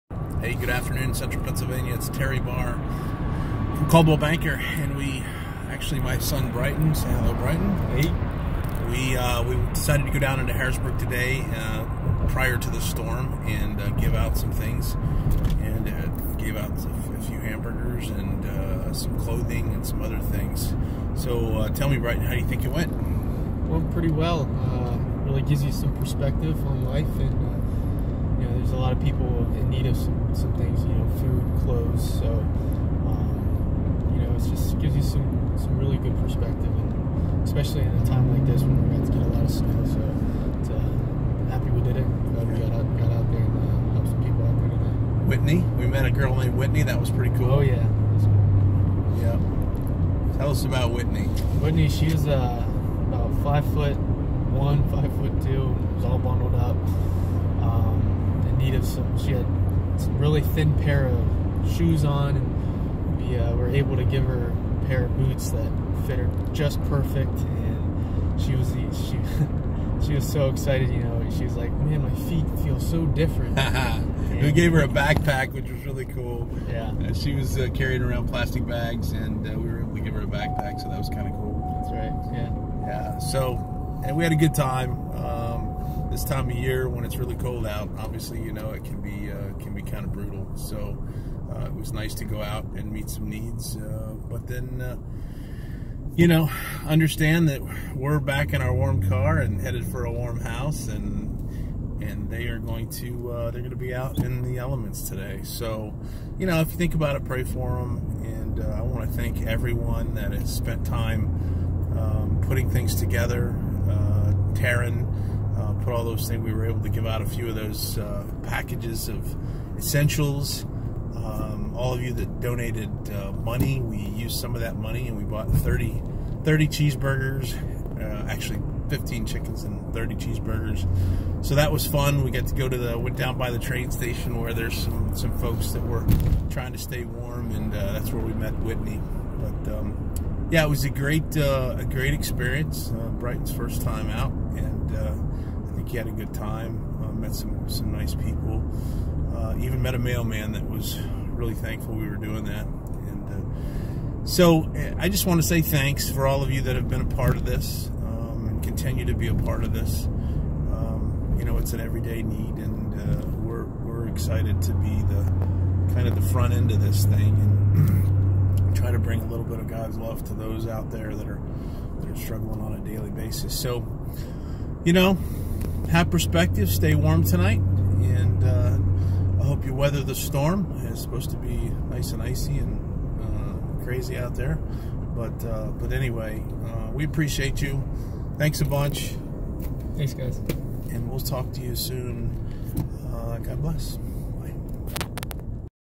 The Barr Group & Helping Hands at Work!
The bar group of Coldwell Banker is helping the homeless in our area those who don't have homes and need a helping hand